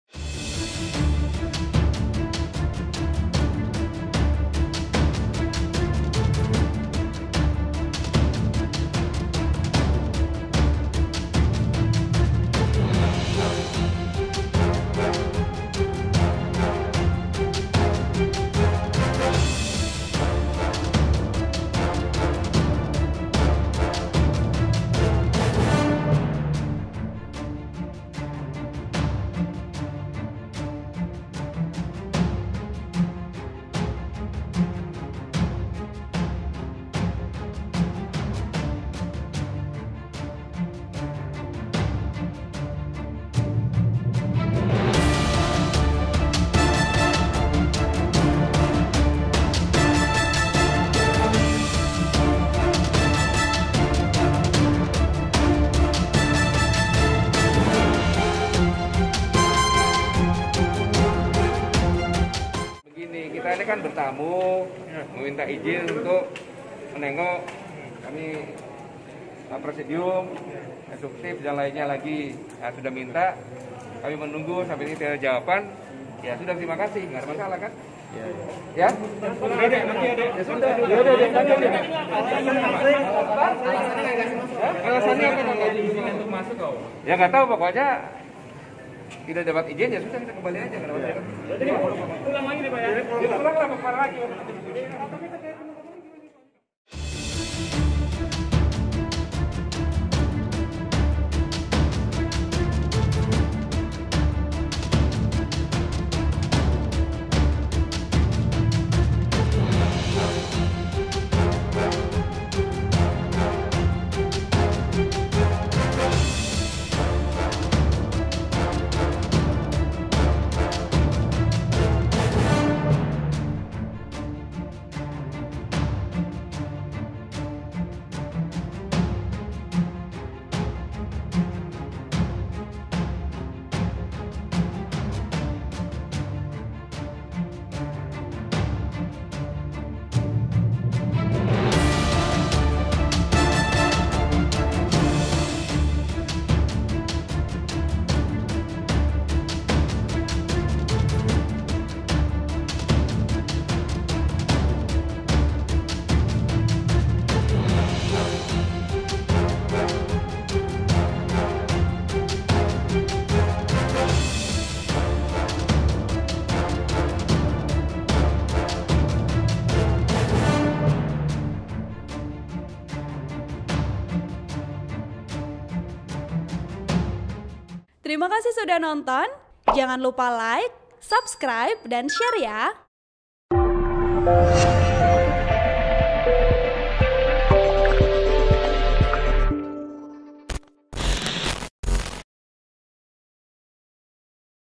Hendak Jenguk 8 Tokoh KAMI yang Ditahan, Gatot Nurmanyto Dkk Ditolak Polisi
Laporan Wartawan Tribunnews.com, Igman Ibrahim

TRIBUN-VIDEO.COM, JAKARTA - Sejumlah tokoh ataupun deklarator Koalisi Aksi Menyelamatkan Indonesia (KAMI) mendatangi Bareskrim Polri, Jakarta Selatan, Kamis (15/10/2020) siang.

Berdasarkan pengamatan Tribunnews.com di lokasi, tampak sejumlah tokoh KAMI yang hadir adalah mantan panglima TNI Jenderal (Purn) Gatot Nurmantyo dan mantan Ketua Umum PP Muhammadiyah Din Syamsuddin.

Selain itu, ada pula tokoh Nahdlatul Ulama (NU) Rochmat Wahab, akademisi Rocky Gerung, dan mantan politikus PPP Ahmad Yani.

Artikel ini telah tayang di Tribunnews.com dengan judul BREAKING NEWS:Gatot, Din, Rocky Ditolak Saat Ingin Jenguk Tokoh KAMI yang Ditahan di Bareskrim,